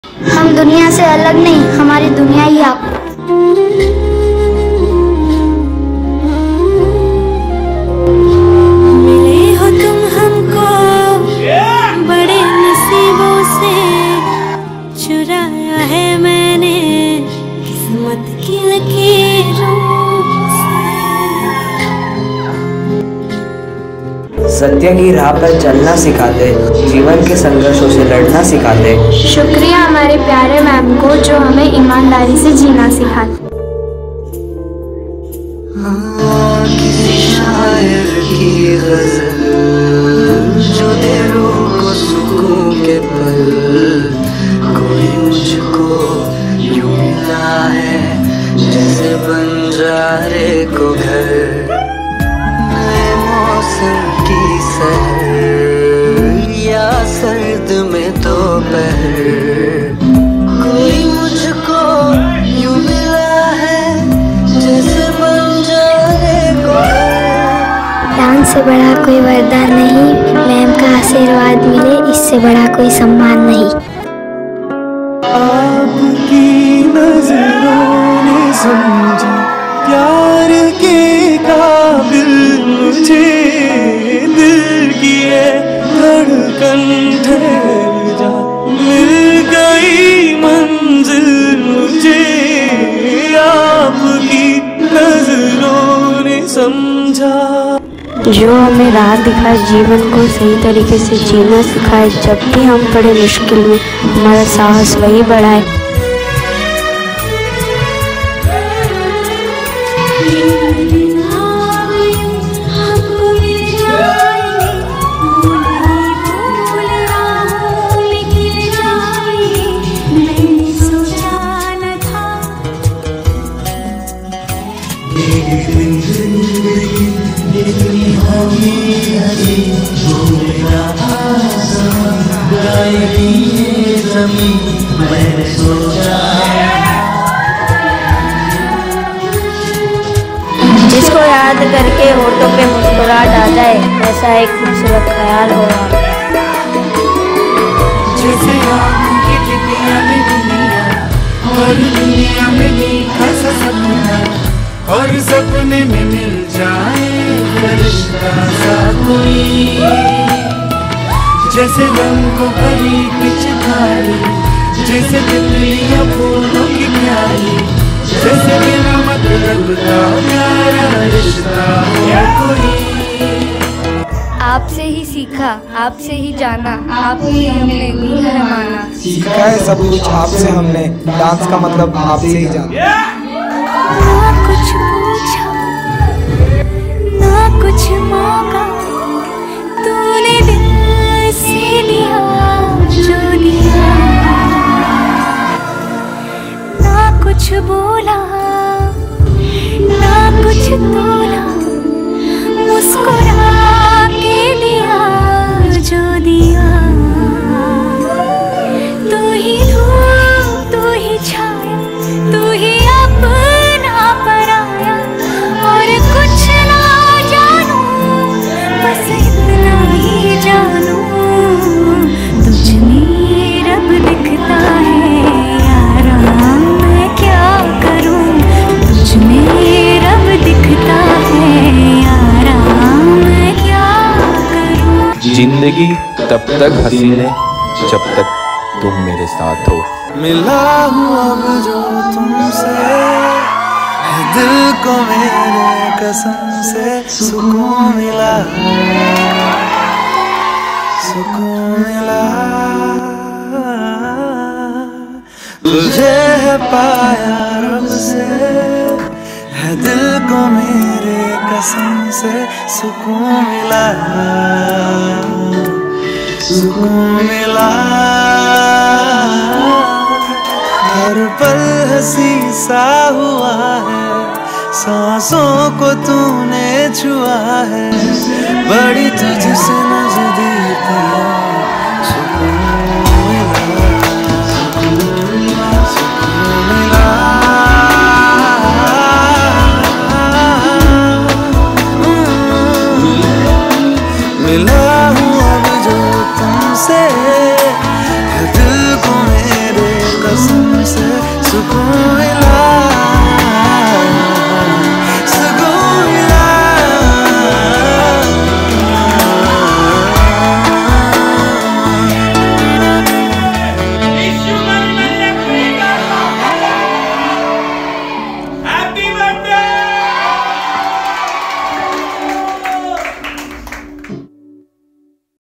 B'day Surprise | World’s Best Mam | Nikita Mayatra | Dance | ABCD Dance Factory | Choreo
B’day Surprise To Our World’s Best Mam @nikitamayatra 😍❤️🔠💕🎉
by ABCD Kids & @yogeshraval27 Sir & Friends💕✨We Love You Mam💕 Thank You For Everything..😍🙏🎉💕

Director / Choreographer
Nikita Mayatra
Yogesh Raval